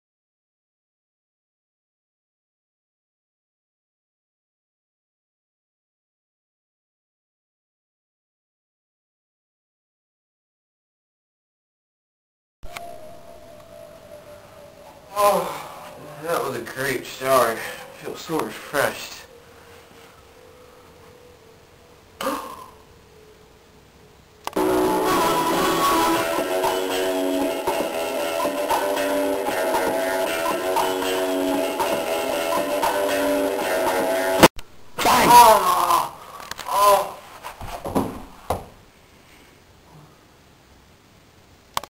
How James Bond Should Kill Bad Guys
You know how a whole movie is about James Bond killing one single bad guy?
Maacinc thinks that if James Bond did it this way it would be a lot easier